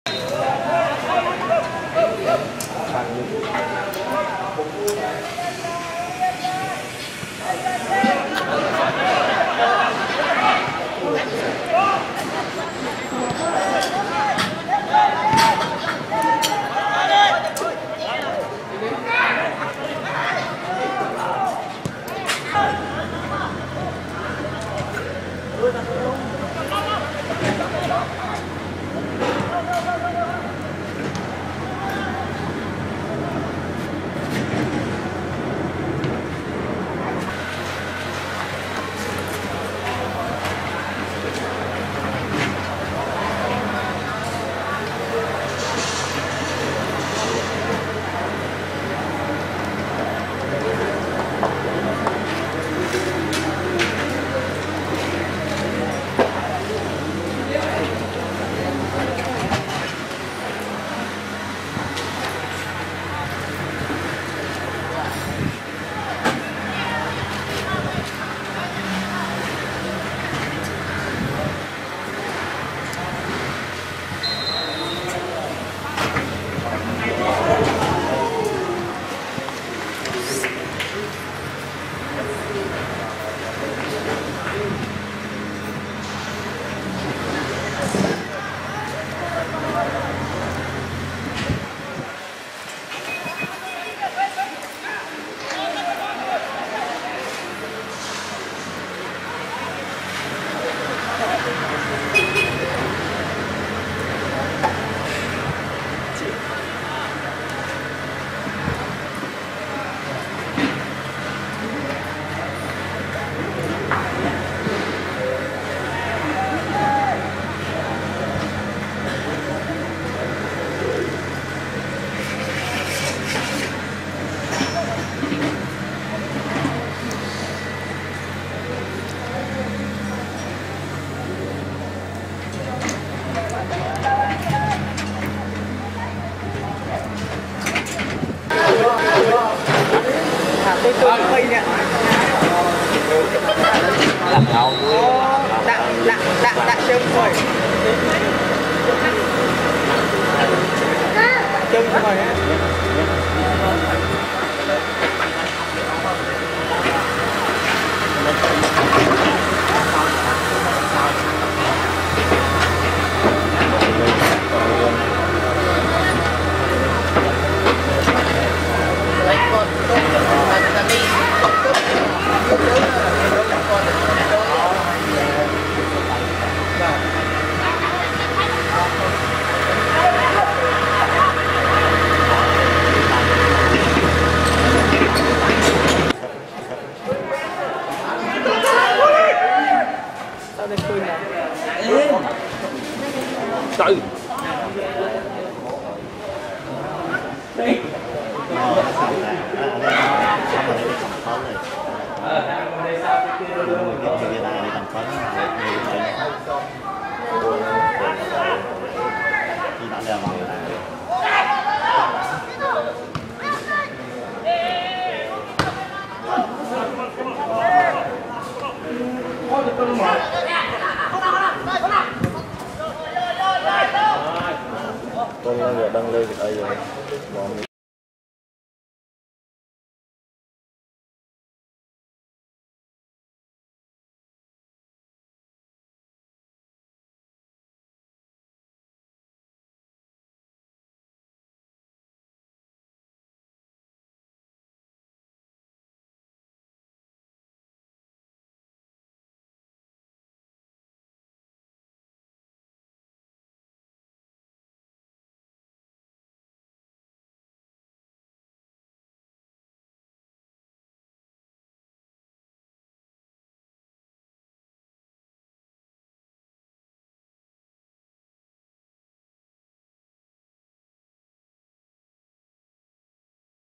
6.3.2015: PPCFC 1 Cambodia National Team 2 (Friendly).
Phnom Penh Crown (in blue) were beaten 2-1 by the Cambodia National Team squad at RSN Stadium, as the NT prepare for their World Cup Qualifying match next week. Santigie Koroma netted a debut goal for Crown just after the half-time break, after goals from Chan Vathanaka and Khuon Laboravy put the NT in front.